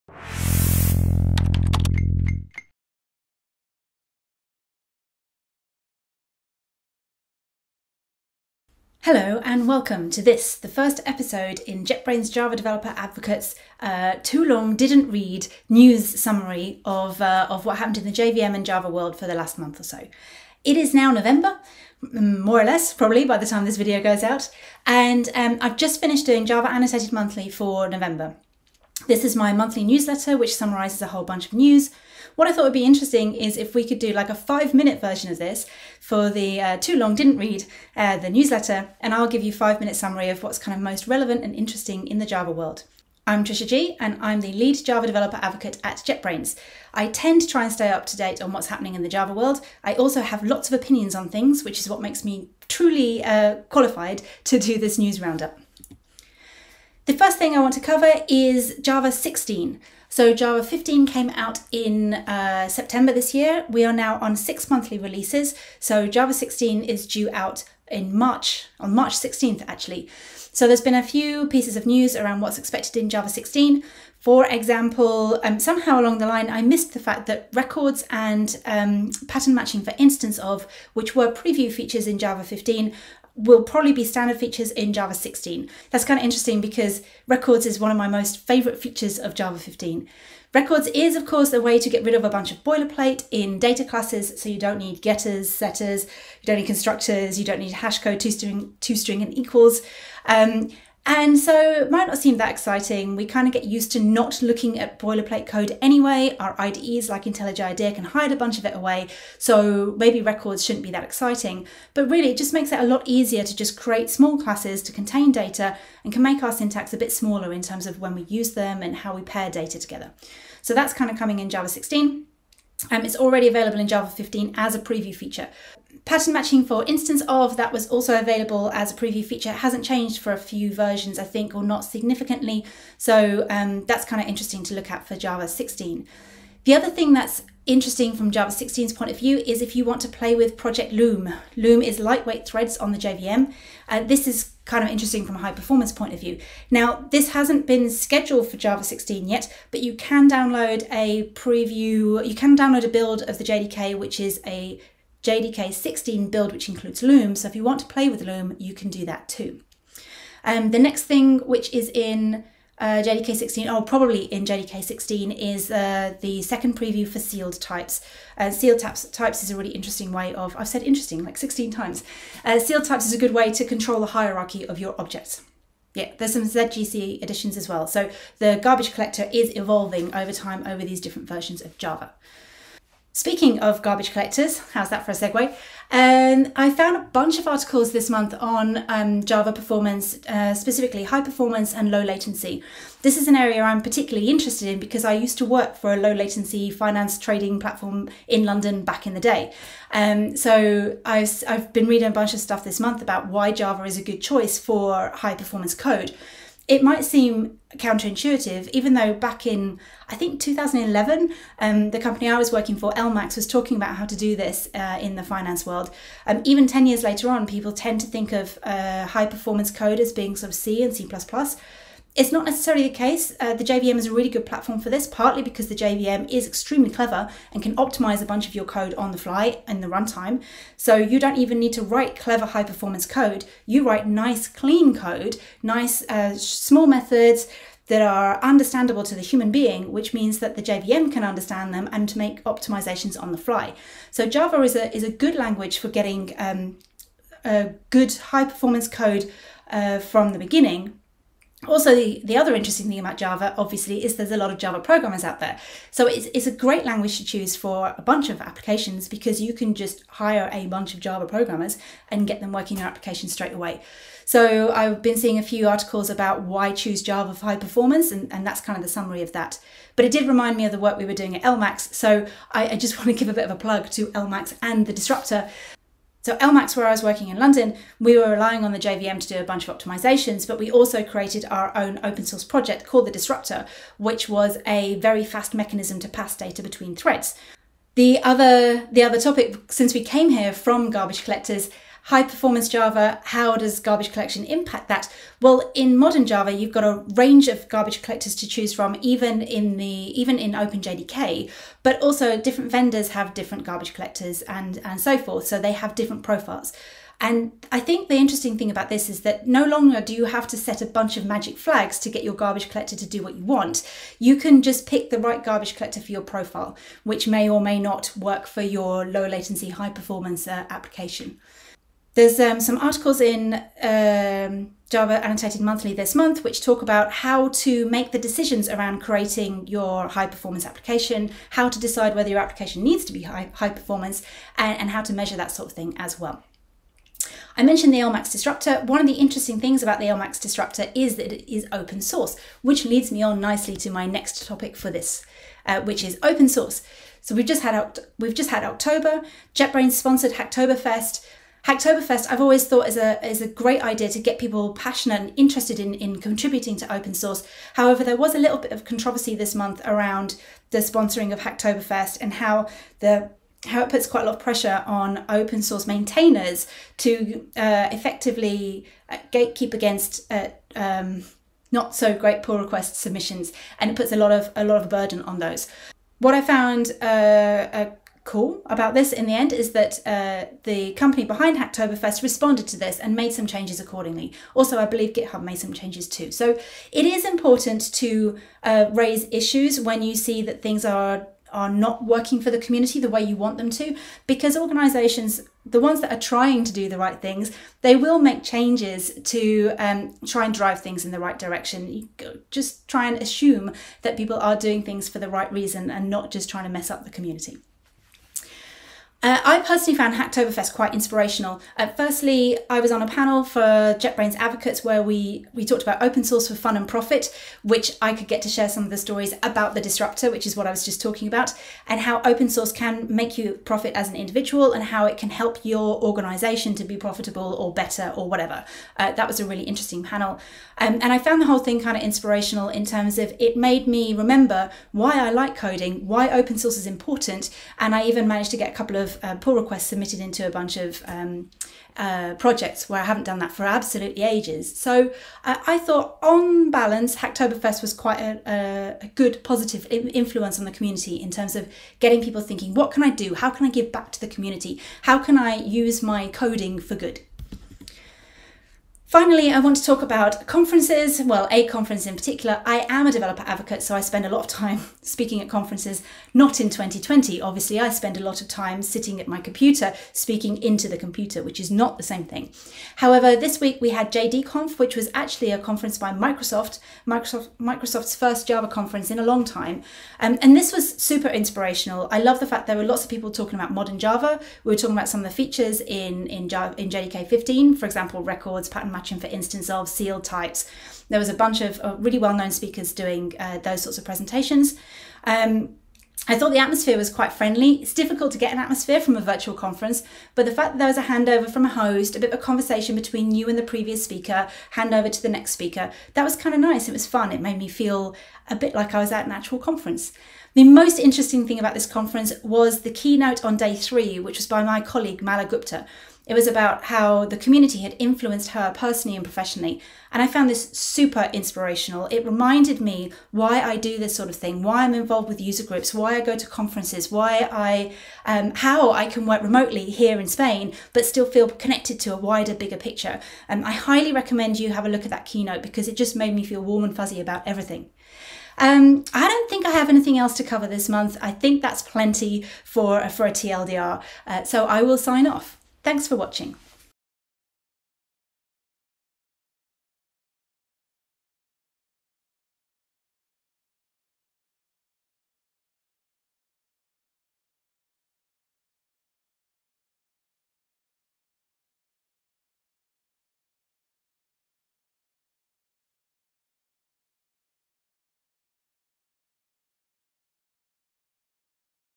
NEWS! November 2020: Java 16, Open Source, conferences and more
This is the very first edition of Java News by Trisha Gee.

Java News!
00:00 - Intro
00:56 - Proposed schedule for JDK 16
01:10 - Finalizing in JDK 16 – Pattern matching for instanceof – this was announced in July, not sure how I missed that both Records and Pattern Matching for instanceof are expected to be “standard” features in JDK 16. So now you know.
02:40 - JEP 397: Sealed Classes (Second Preview) – doesn’t seem to be scheduled for JDK 16 right now, but I expect it will be
05:44 - Open Source
07:39 - Probably the biggest source of Java news was from Microsoft, of all people, this month. They ran JDConf with loads of great talks